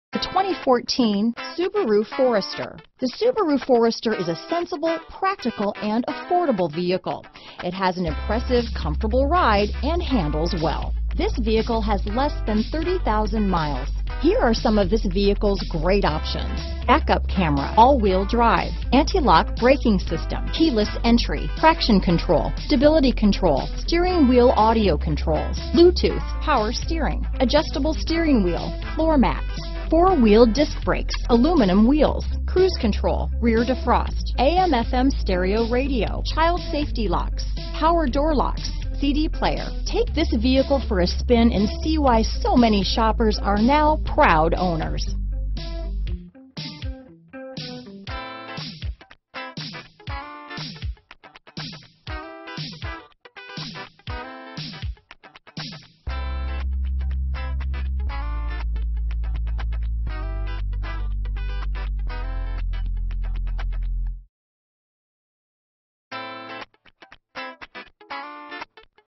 2014 Subaru Forester Troy, Colonie, Glenville, Clifton Park, Saratoga Springs, NY U68317A
Crystal Black Silica Used 2014 Subaru Forester available in Troy, New York at Carbone Subaru of Troy. Servicing the Colonie, Glenville, Clifton Park, Saratoga Springs, NY area.
2014 Subaru Forester 2.5i Premium - Stock#: U68317A - VIN#: JF2SJAEC6EH522172

Some of our Pre-Owned vehicles may be subject to unrepaired safety recalls. Check for a vehicle s unrepaired recalls by VIN CAMERA, ALL WHEEL DRIVE, HEATED FRONT SEATS, BLUETOOTH, MOONROOF, MP3 Player, KEYLESS ENTRY, 32 MPG Highway, ALLOY WHEELS.